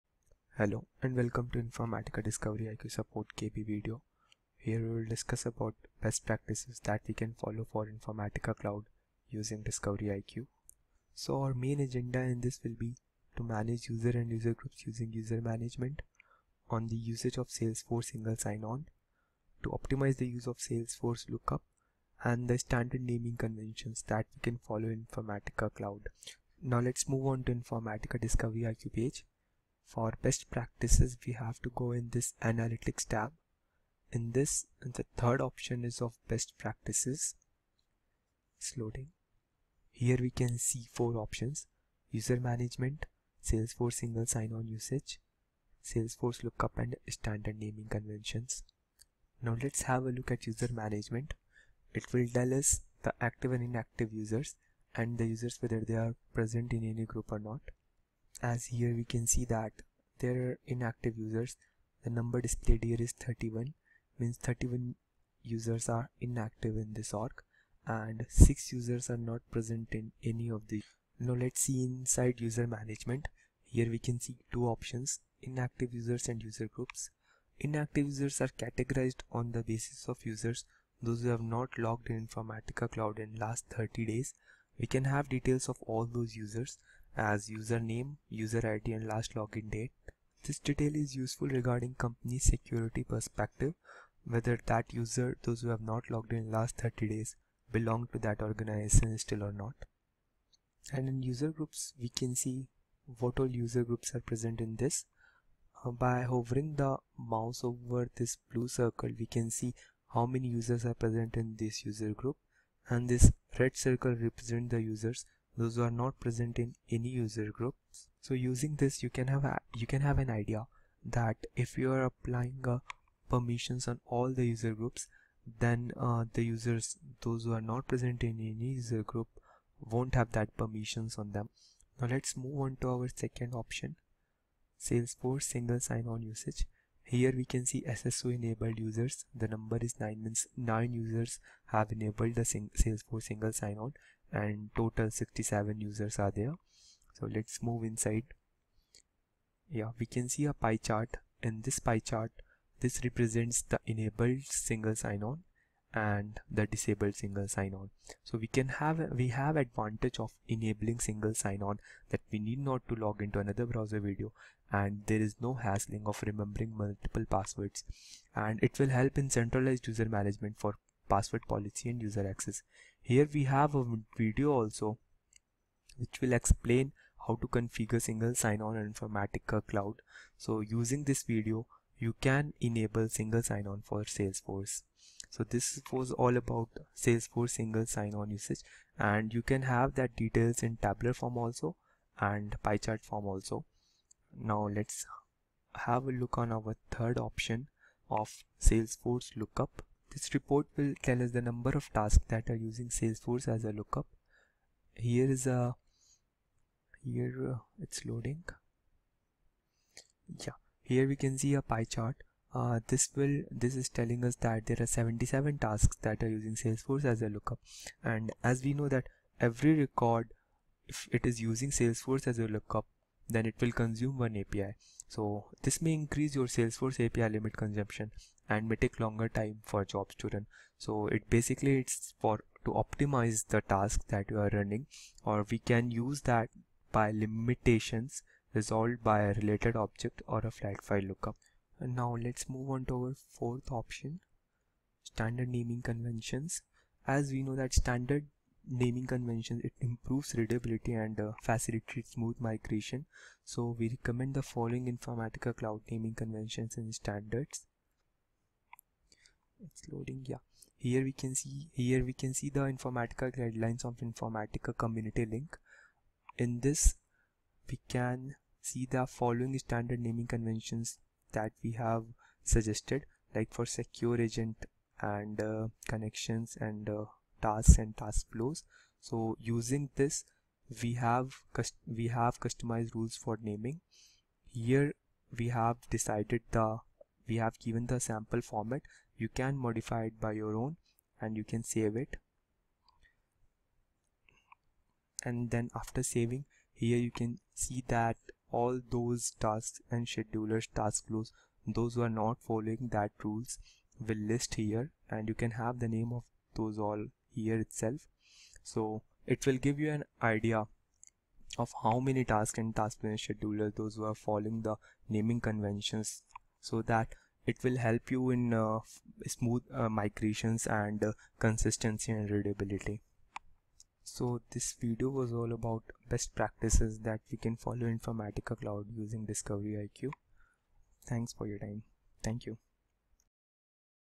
How to follow Informatica Cloud Best Practices using DiscoveryIQ
Agenda of this video includes:

User Management
Salesforce Single Sign-on Usage
Salesforce Lookup
Standard Naming conventions